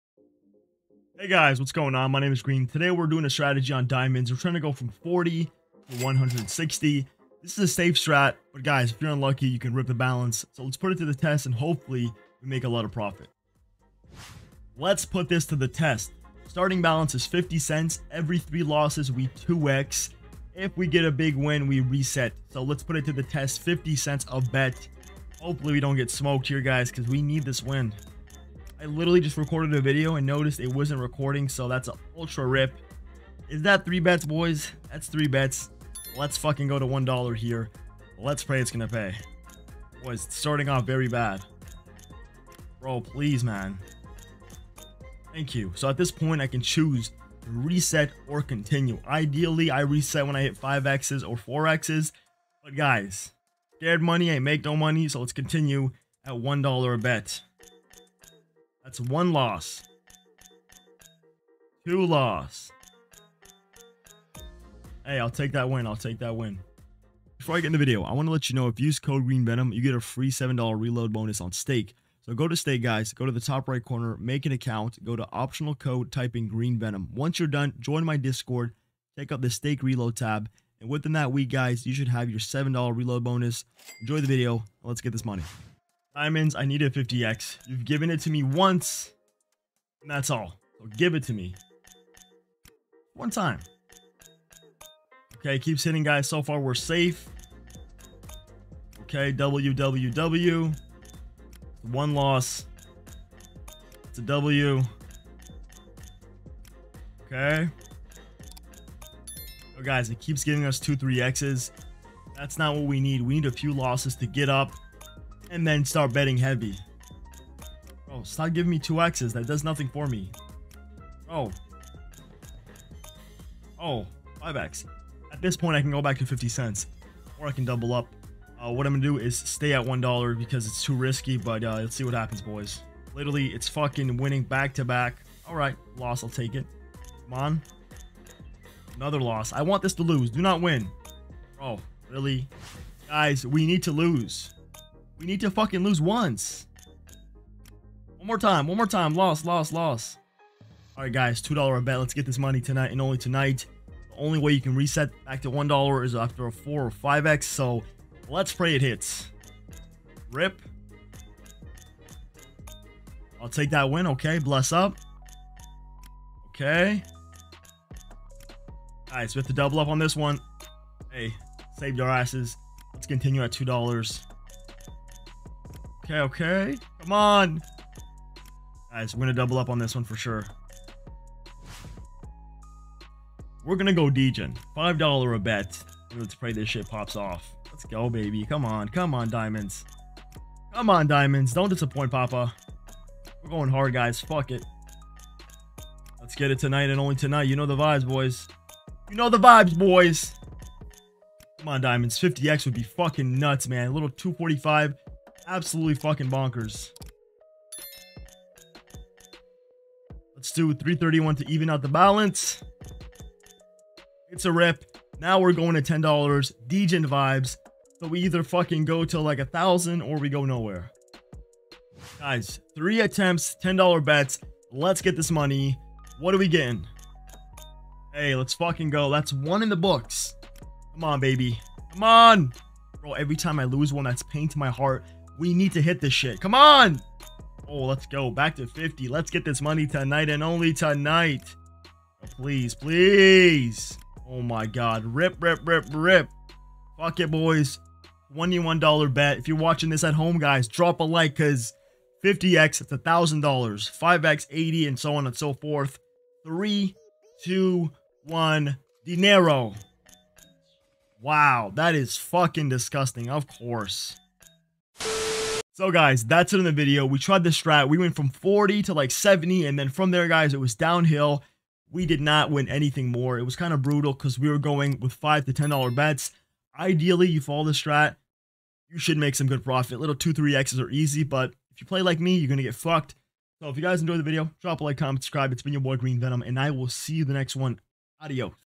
Testing "DEGEN" STRATEGY ON DIAMONDS! (Stake)
Testing an insane strategy on stake diamonds, high risk, fast profit!

💎 Code: "GreenVenom'" on Stake for VIP Rewards & Rakeback + 7$ BONUS